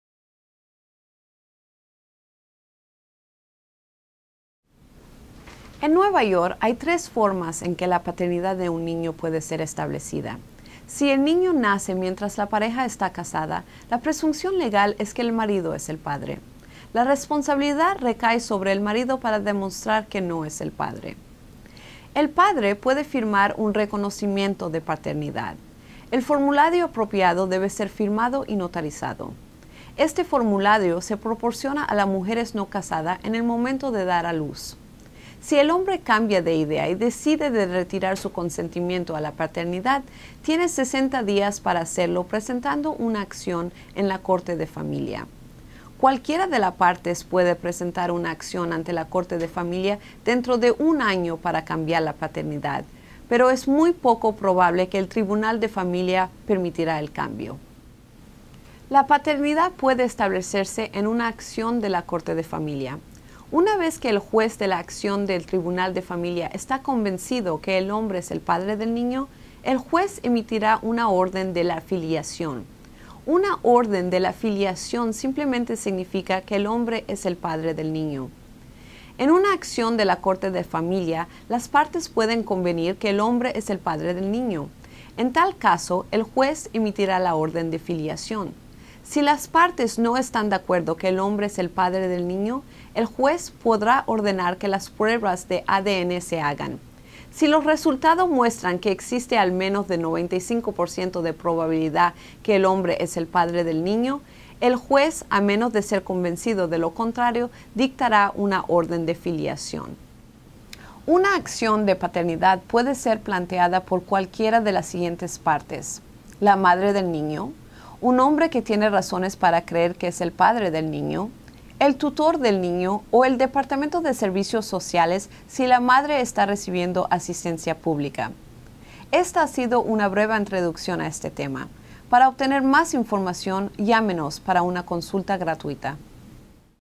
Paternidad en Nueva York (Abogado de divorcio en Brooklyn) (FS8)
La oficina legal de Jeffrey B. Peltz, P.C. proporciona información sobre el tratamiento de las cuestiones de paternidad en la corte familiar de Nueva York.
Para obtener más información, visite nuestra página web:
AAALawyer.com
NewYork-DivorceLawyer.com